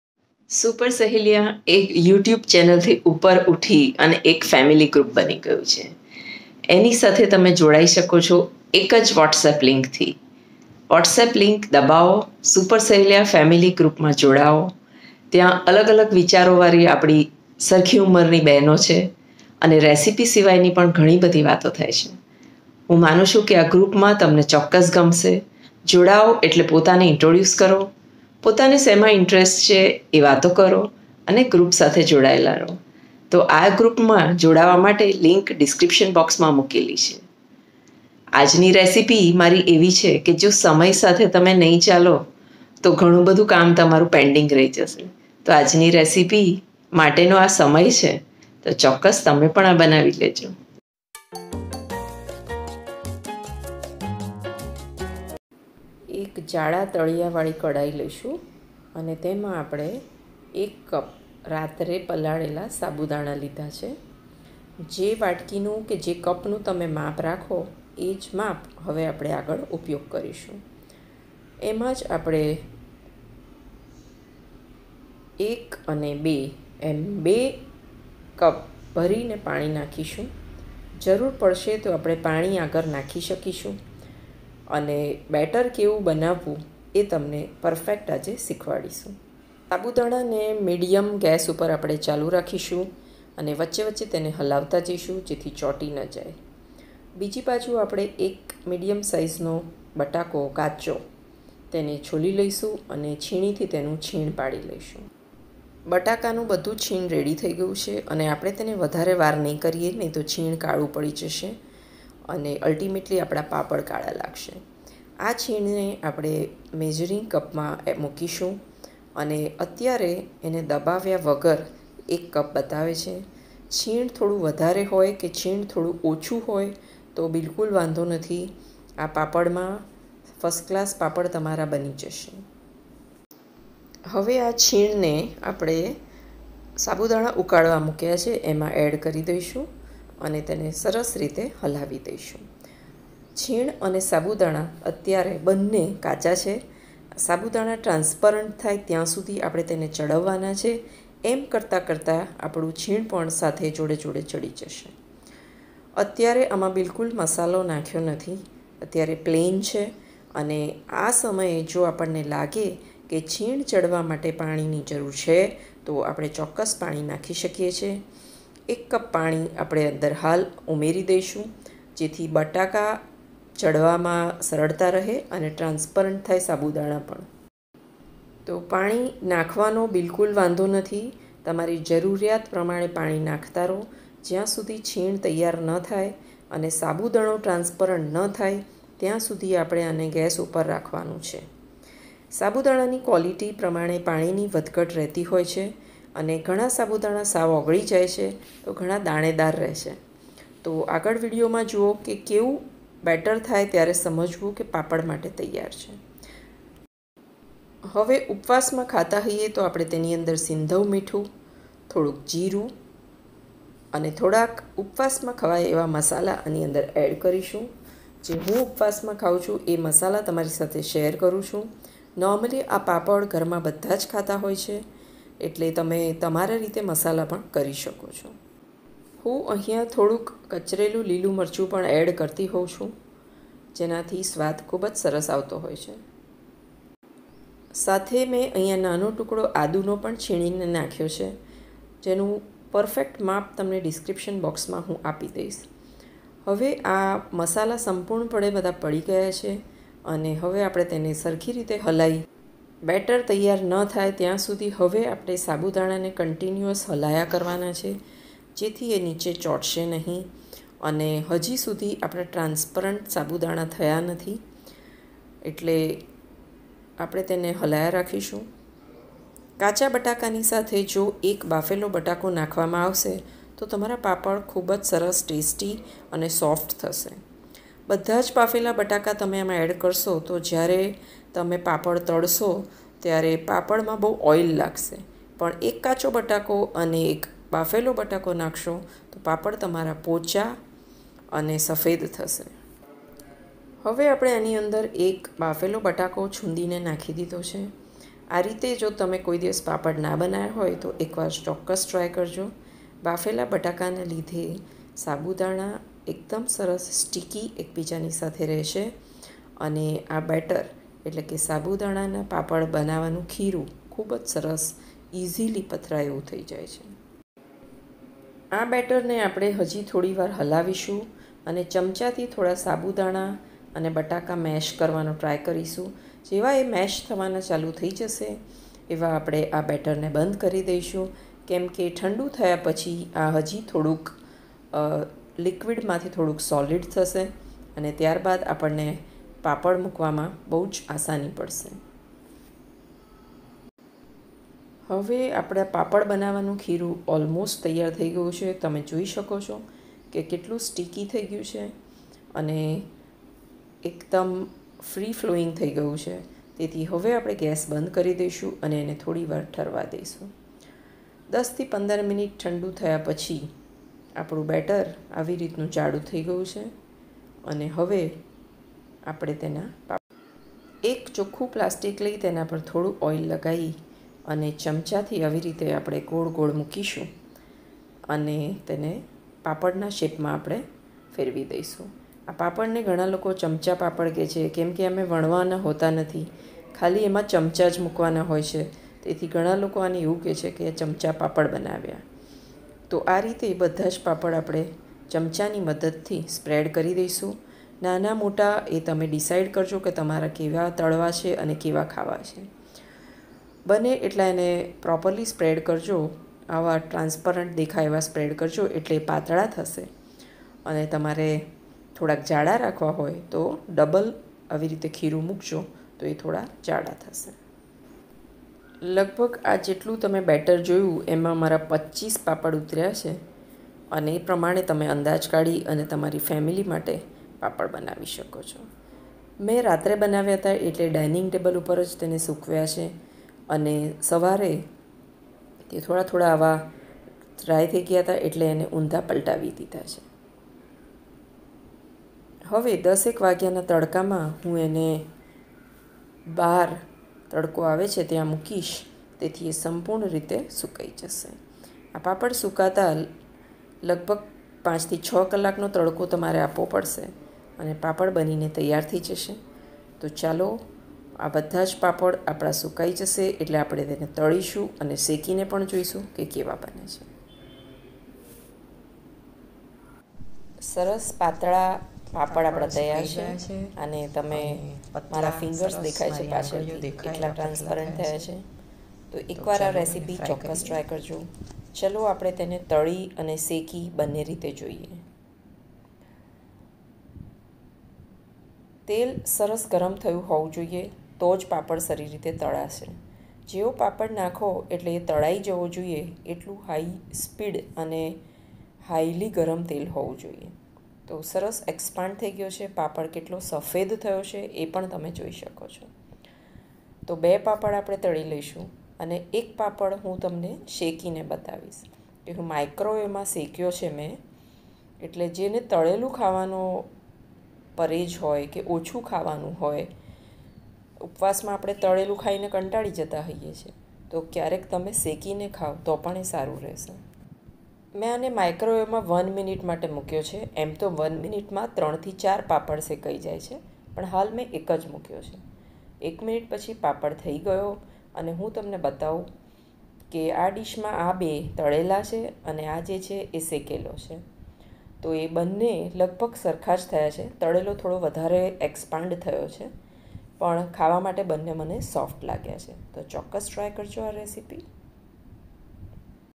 ઉનાળા ના બધા કામ બાજુ માં મૂકો પેહલા આ કામ કરો || ઘઉં ભરવા,મસાલા ભરવા,રસ ભરવો તો થશેજ પણ આ રહી જશે
India is a country of festivals and ceremonies,because we indians feel that there should always be balance between work and happiness.festivals gives us a break from our routine work and fills us with energy and power.Along with celebration we also observe fast in some festivals.
Today from the kitchen of" SUPERSAHELIYA".i myself naimisha patel is there before you to share my recipe of bataka na papad .so lets check ingredients

sago(soaked for 5 hours) 1 bowl
potato grated 1(  raw and medium sized)
potato 1 (small and boiled)
water 4 cups( depends on quality of sago)
crushed chillies ૨ tsp
salt ૨ tsp
jeera 1tsp
ginger grated 1/2 inch

check  full video for proper understanding of making papad.